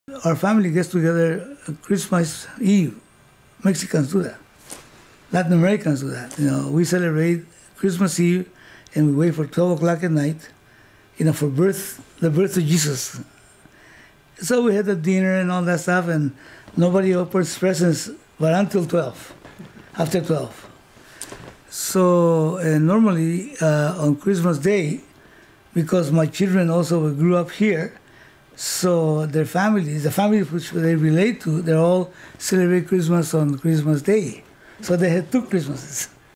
Family Christmas with Luis
In this video Luis tells a little about his family's Christmas traditions.
Produced for Voices of My Heritage by Terry Lamb of Videosyncracy